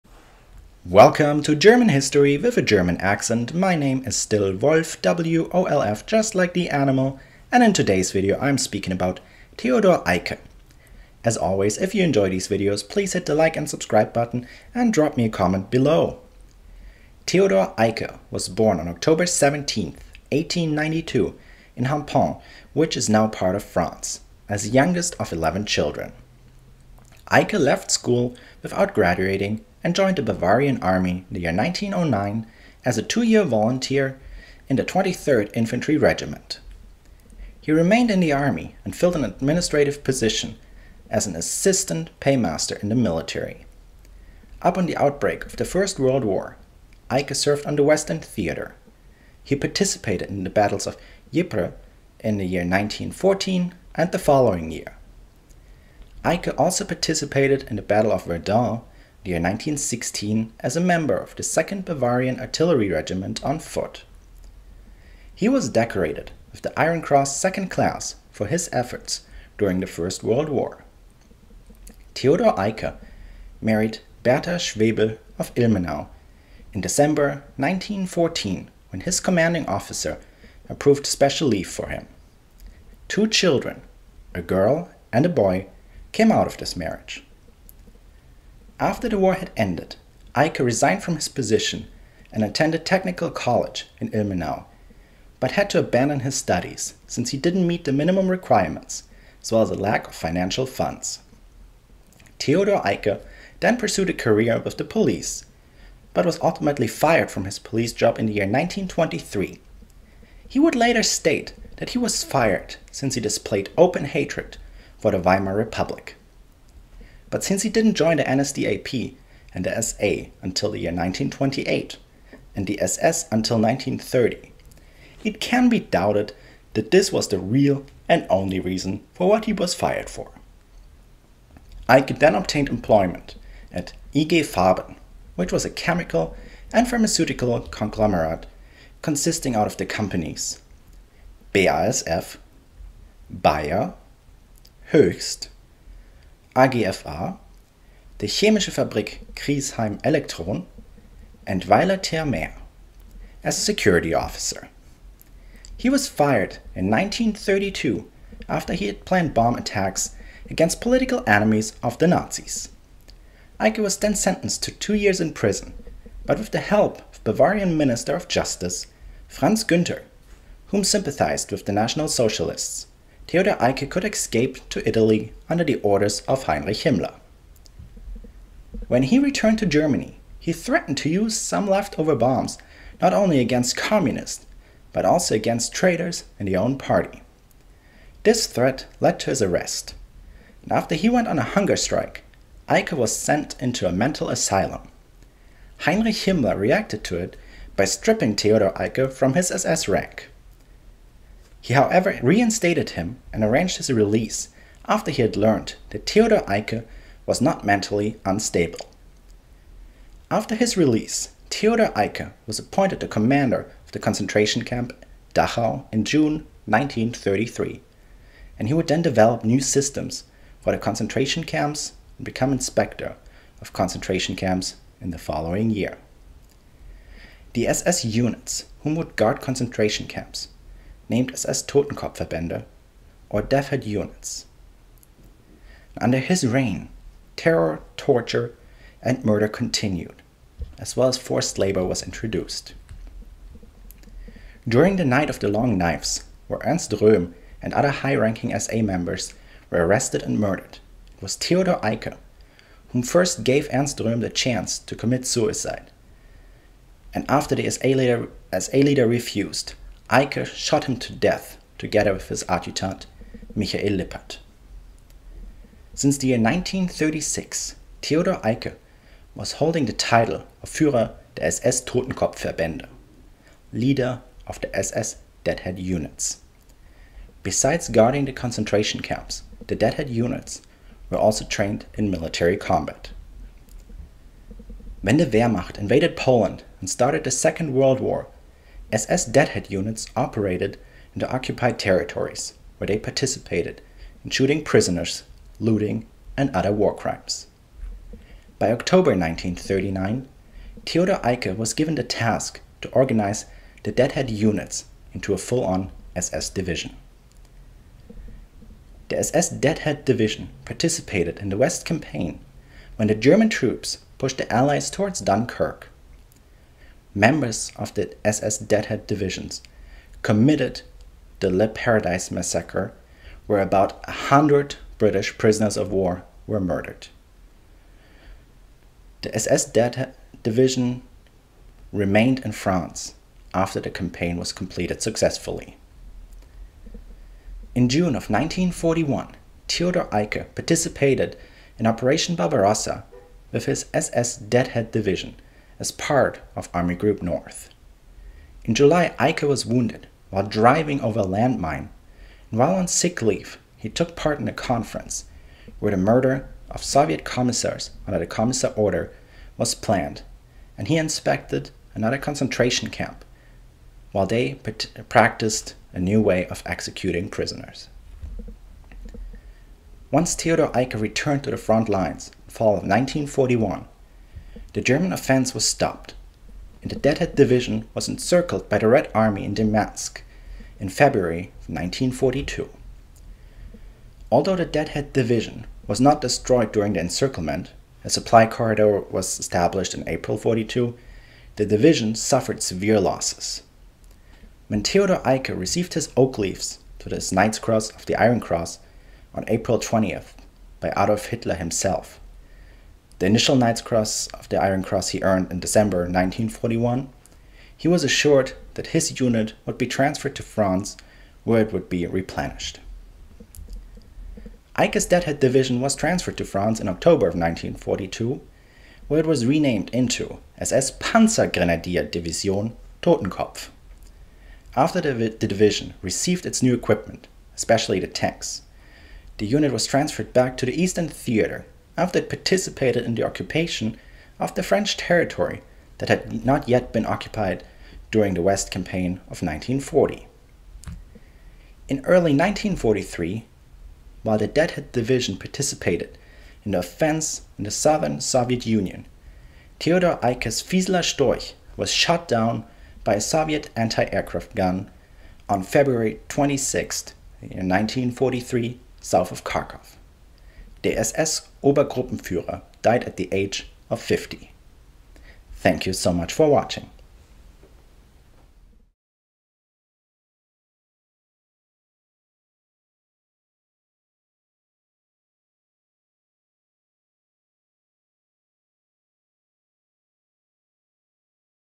Who was Theodor Eicke? (English)
A short video about Theodor Eicke (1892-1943)

Federal German Archive
Milenioscuro
Furfur
Sith Cookie
Cmapm
Memnon335bc
Eric Gaba